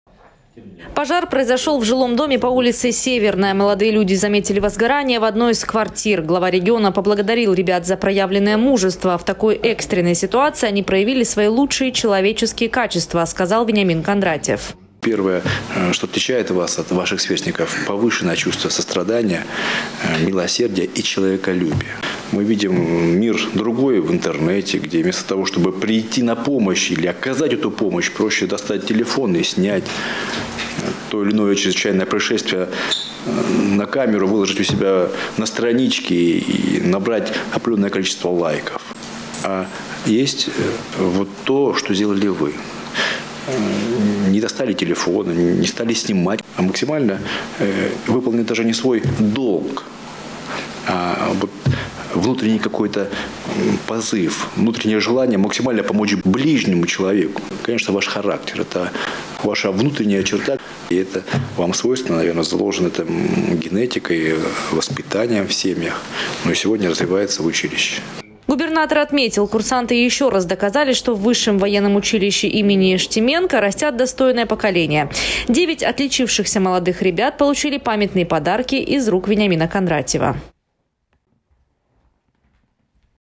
Губернатор встретился с курсантами военного училища, спасшими людей из горящего дома. Эфкате Сочи
Глава региона поблагодарил курсантов Краснодарского высшего военного училища за проявленное мужество. В такой экстренной ситуации они проявили свои лучшие человеческие качества, подчеркнул Вениамин Кондратьев.